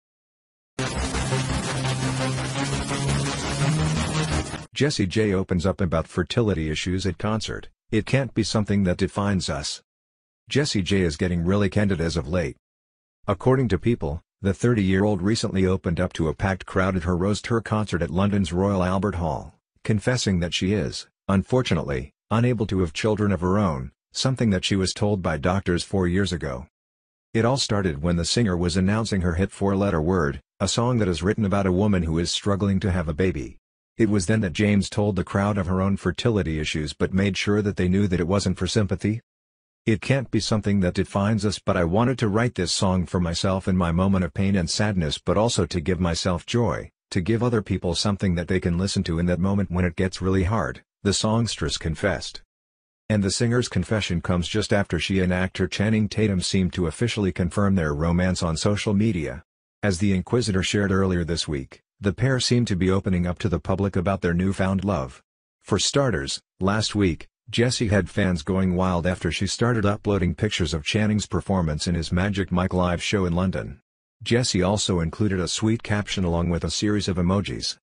JESSIE J TALKS ABOUT HER FERTILITY ISSUES: ''IT DOESN'T DEFINE ME''
Jessie J talks about her fertility Issues: ''It doesn't define me''  Jessie J confessed about her Fertility problems at a Concert at London's Royal Albert Hall and talked about that with the crown, encouraging people with the same problems to now be afraid!